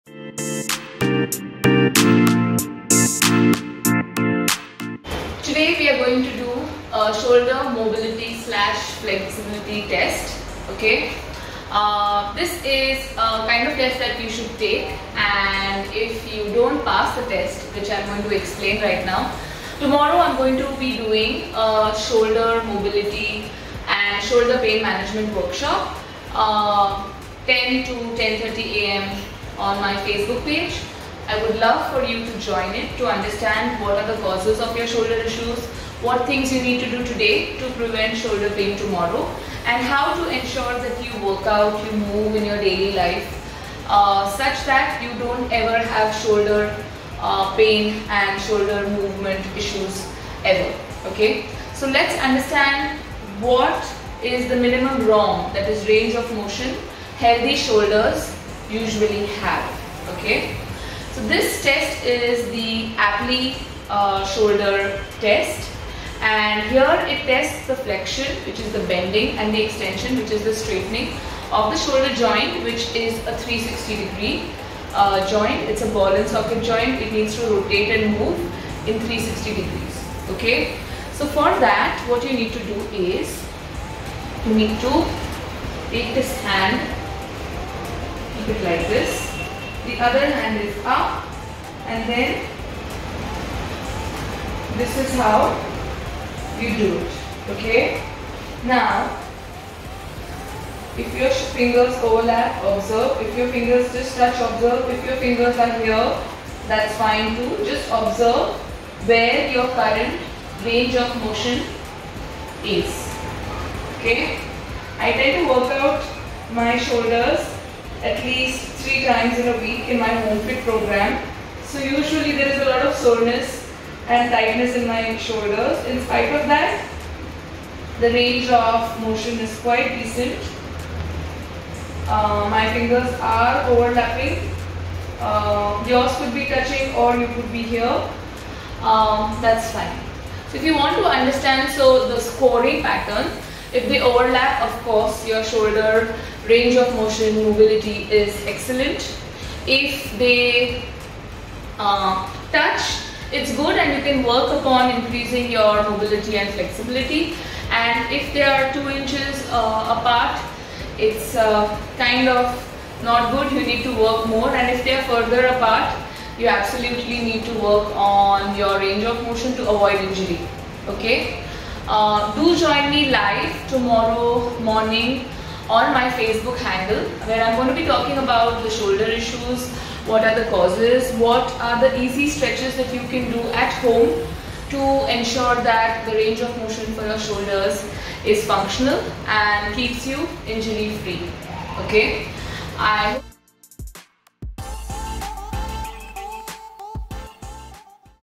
Shoulder strength and flexibility test
Being fit is addictive! Do you have strong and flexible shoulders? Here is test to confirm just that.

Tell us how you performed...
Join our free workshop Shoulder pain management workshop (30 min) to improve your shoulder strength and flexibility